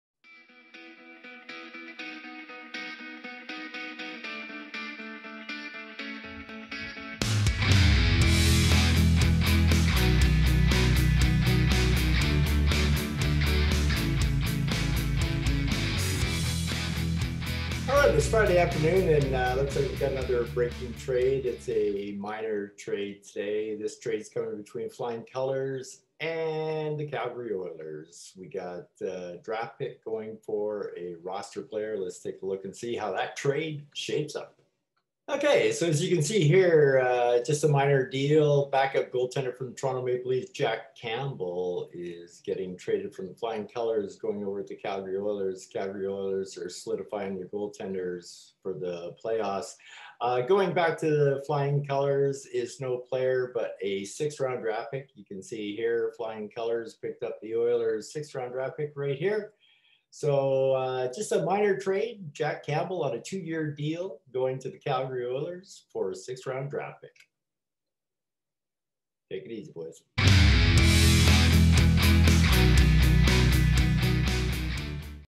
FC:Oilers Trade
Back up goaltender for a draft pick